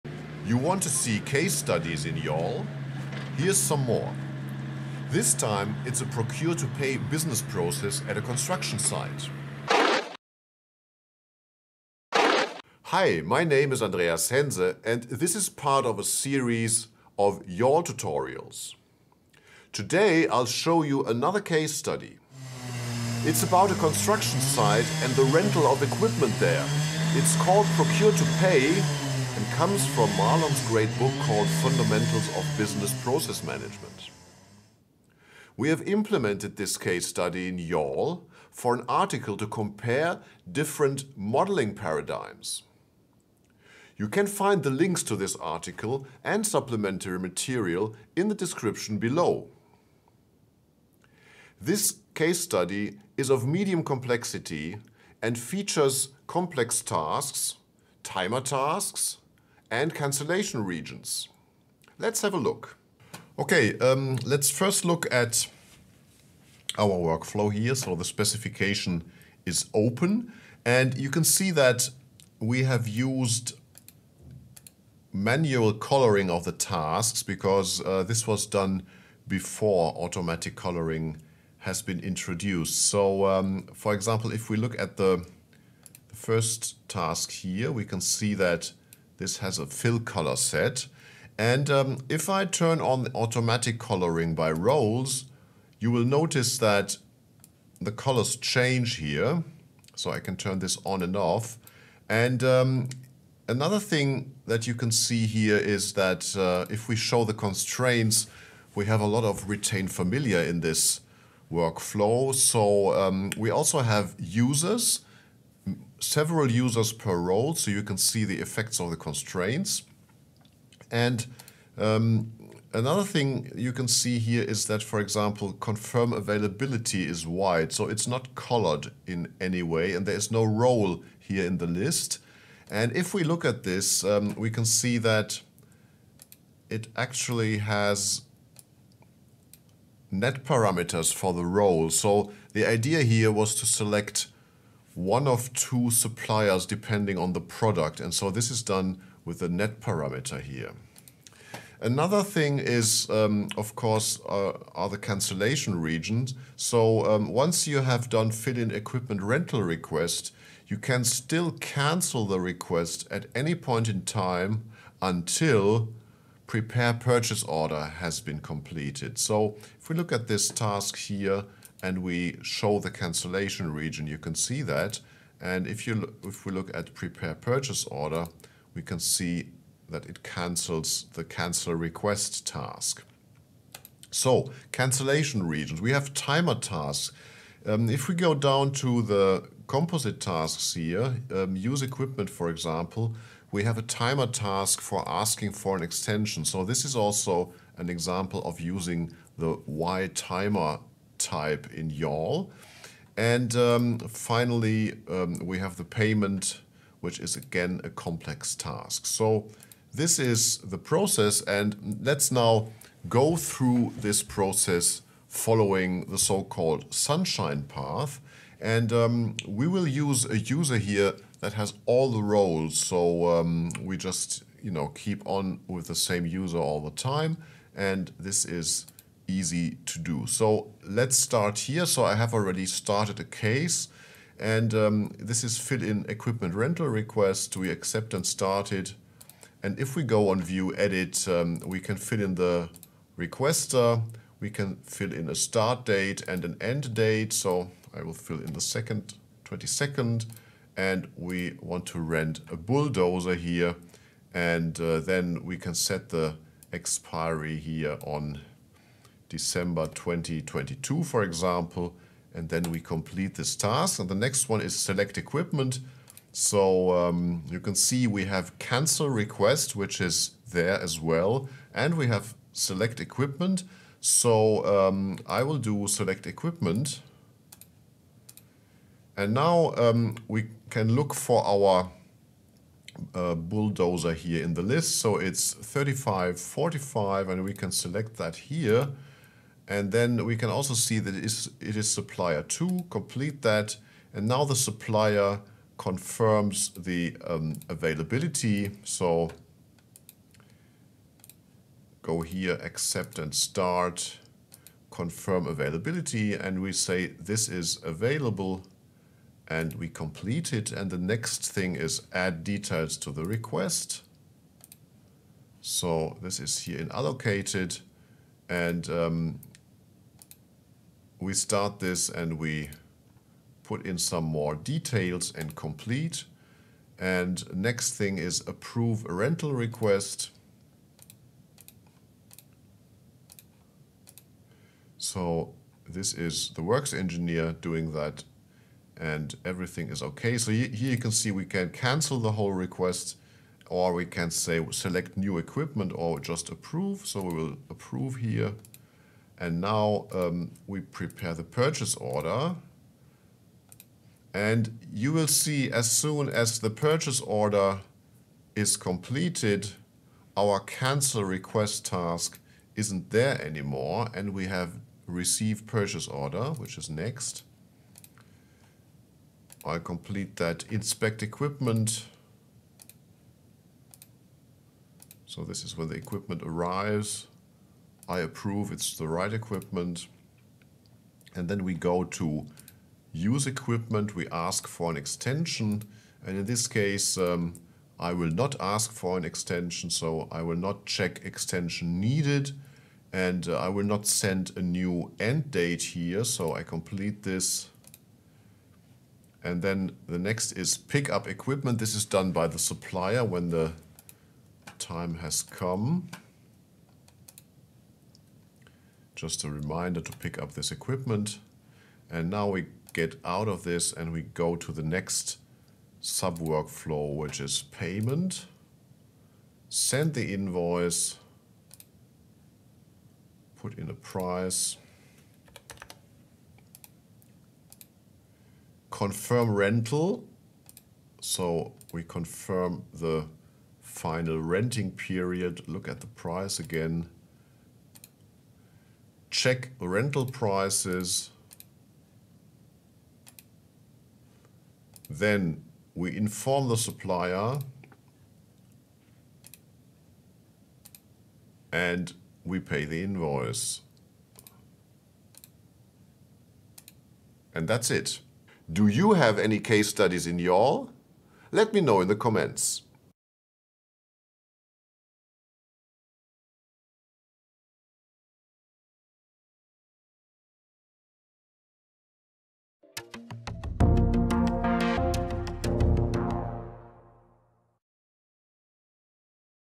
YAWL Workflow Case Study: Procure to Pay
Procure to Pay - or P2P - is a YAWL case study that comes from "Fundamentals of Business Process Management" - see below - and that we have implemented in YAWL  for the article
This case study is of medium complexity and features complex tasks, timer tasks, and cancellation regions.
Workflow Automation Examples

This tutorial was made with YAWL V4. In YAWL V5, the forms have changed completely. Nevertheless, the case study works the same.

00:00 Intro
00:27 Case Study
01:02 P2P Workflow Specification
03:32 Walkthrough Sunshine Path
07:38 Conclusion